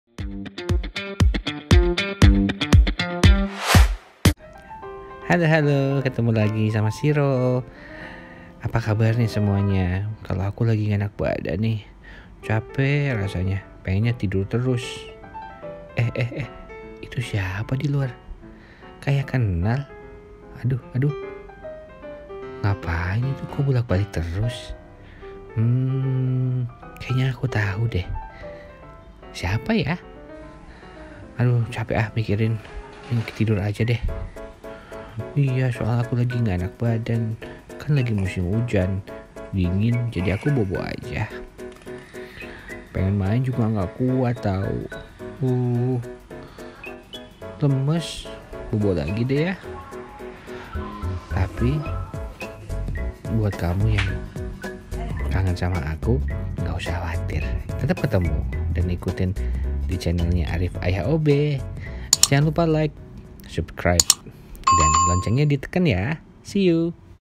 SiRO feel not Well [siro lagi ga enak badan ]
SiRO was not feeling well since yesterday !
He slept whole day and not really active, but he eat quiet much! based on my experience, I give him @RoyalCaninRecovery and today he look more stronger than before . I hope he will recovery soon!